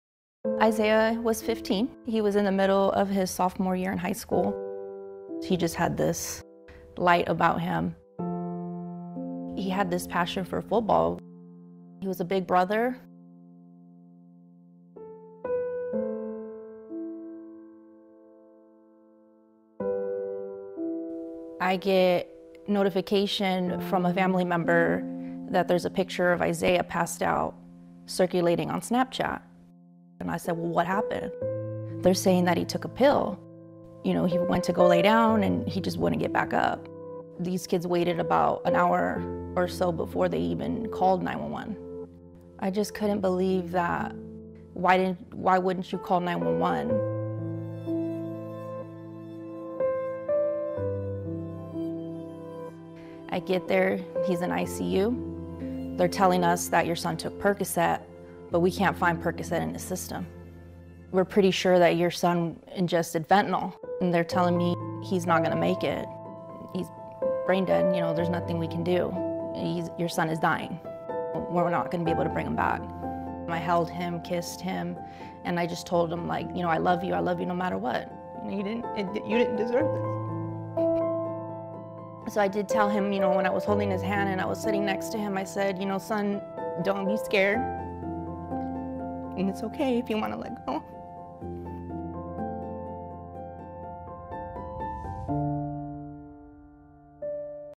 Issaiah's Story
Story of teen Issaiah and how his life was taken by fentanyl with one pill.